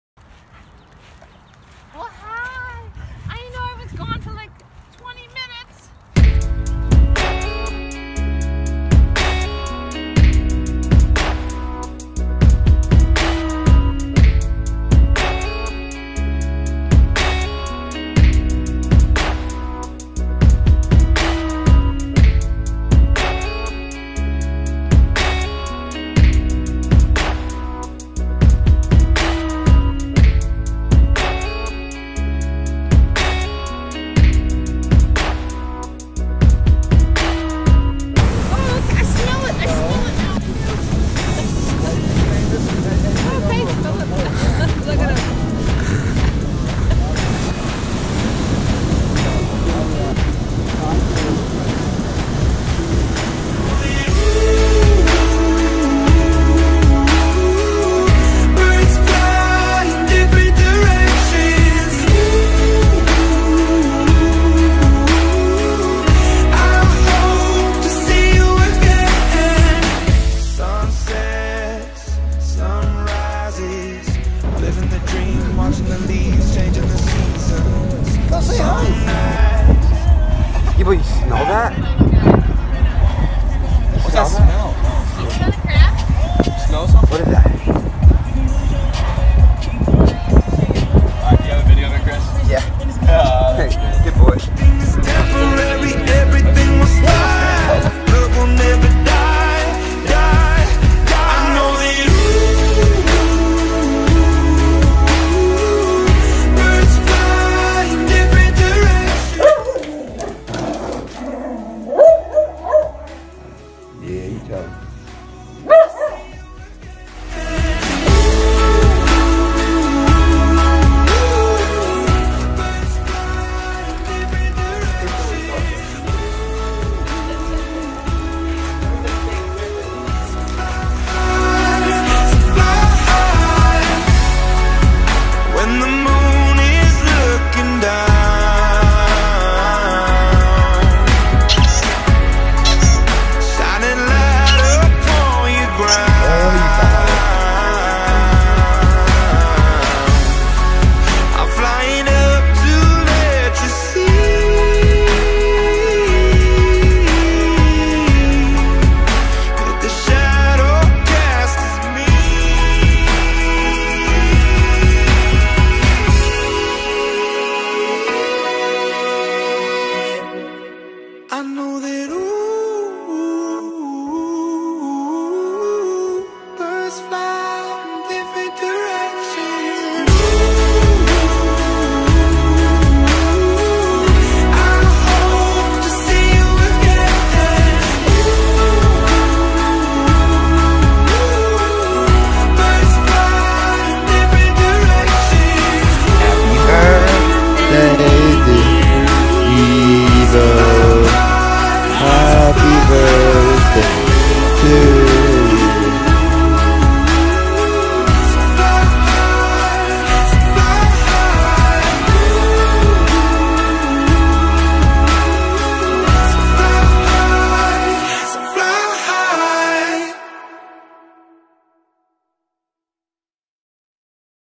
For Skibo
This video is a celebration of Skibo's wonderful life and all the amazing memories we made with him